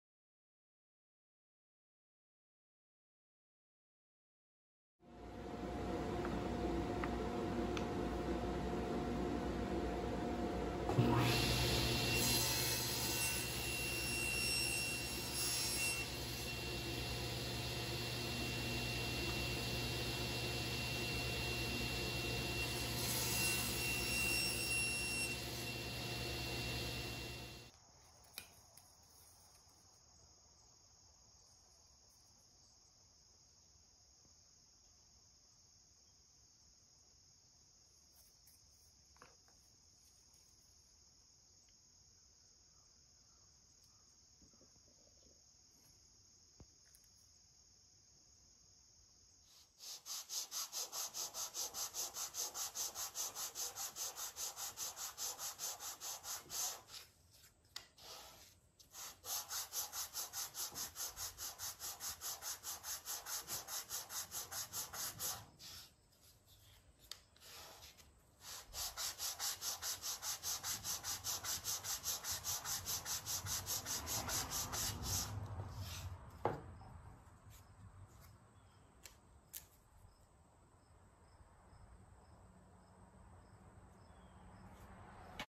How I make end grain veneer
This is a quick overview of how I make endgrain veneer. It’s important to use a sharp, thin-kerf blade in your saw.